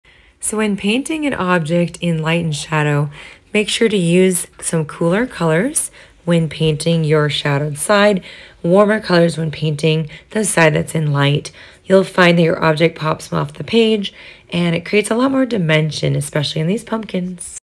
Paint watercolor pumpkins with warm and cool colors for beginners
Hi guys! Here’s a quick tip on how to produce light and shadow on your objects by using warm and cool paint colors.

❤️ My newsletter: Learn about painting and self-care tips, promos and sales, and when I share a free tutorial!